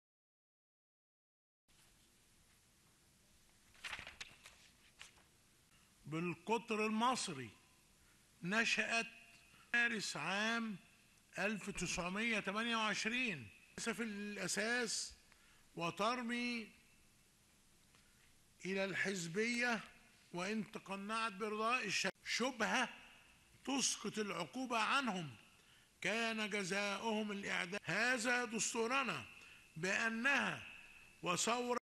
Egypte: prison à vie pour l'ex-président Morsi pour "espionnage"
Un tribunal égyptien a condamné mardi l'ex-président islamiste Mohamed Morsi, destitué en 2013 par l'armée, à la prison à vie pour "espionnage" au profit du Hamas palestinien, le Hezbollah libanais et l'Iran.